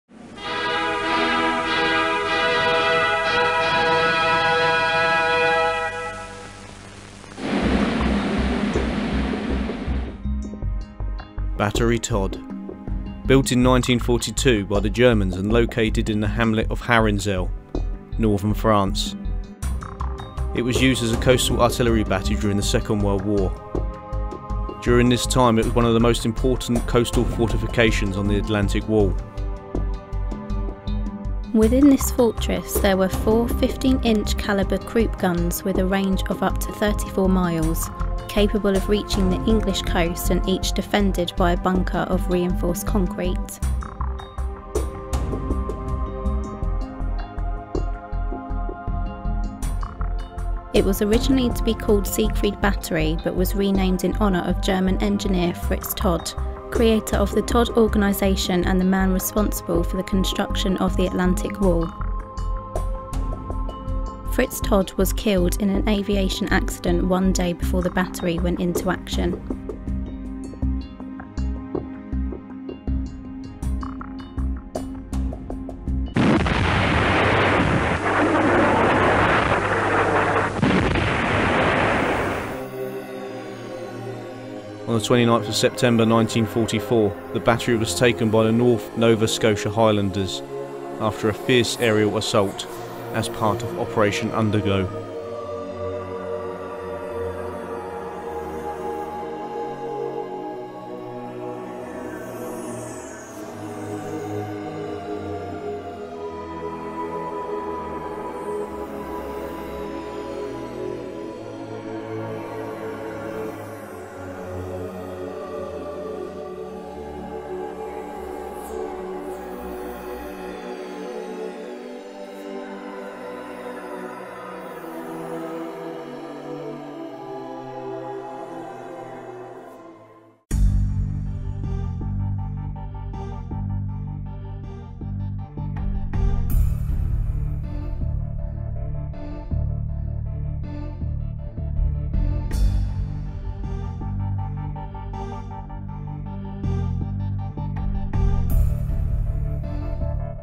BATTERIE TODT -- GERMAN BUNKER Series -- HITLERS ATLANTIC WALL
This is the introduction into the German gun Battery TODT Episode 2 into the German Bunker Series.